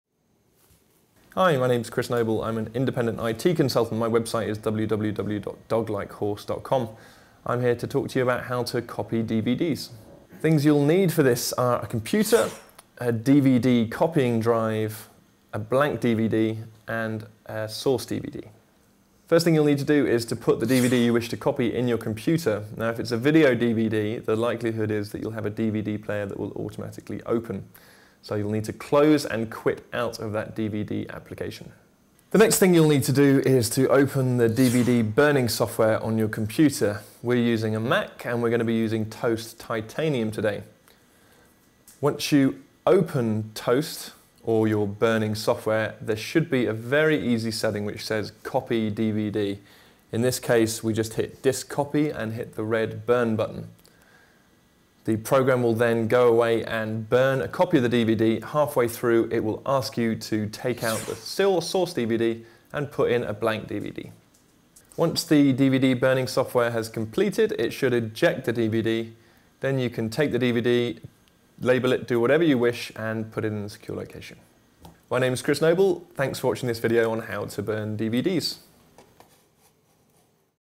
Computer Basics : How to Make Copies of DVDs
To make copies of DVDs, you'll need a DVD copying drive, a blank DVD and a source DVD. Find out how you can make copies of your DVDs from an IT consultant in this free video on computer storage.

Bio: Christopher Noble is an IT entrepreneur who was instrumental in building two successful IT companies.